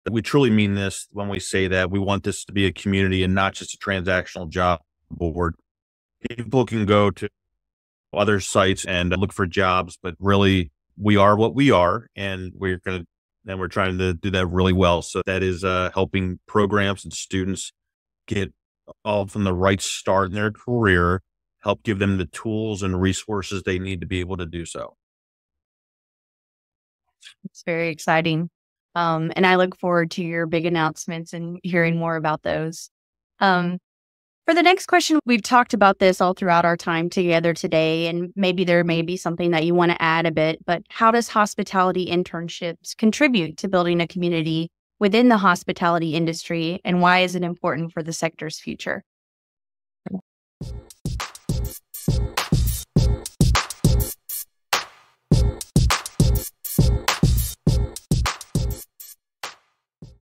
Building a True Community, Not Just a Job Board | INNOVATION Insights LIVE
In this episode, we discuss the importance of fostering a community rather than just providing a transactional job board for the hospitality industry. Hospitality Internships aim to support programs and students from the start of their careers by offering the necessary tools and resources. Additionally, we explore how Hospitality Internships contribute to the industry's community-building and their significance for the sector's future.

Discover exclusive gift items, empowering gift cards, transformative seminars and workshops, and free creative coloring pages.